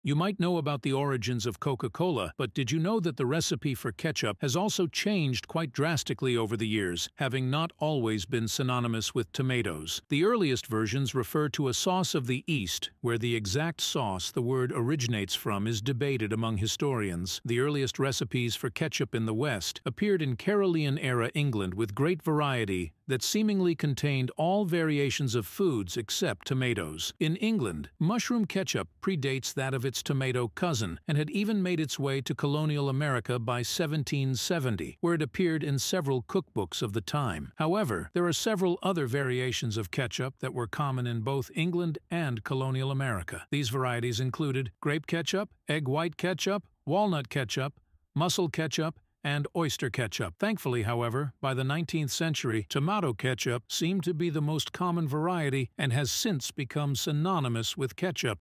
You'll Never Look at Ketchup the Same Again... A History of Ketchup
The long and kinda gross history of ketchup...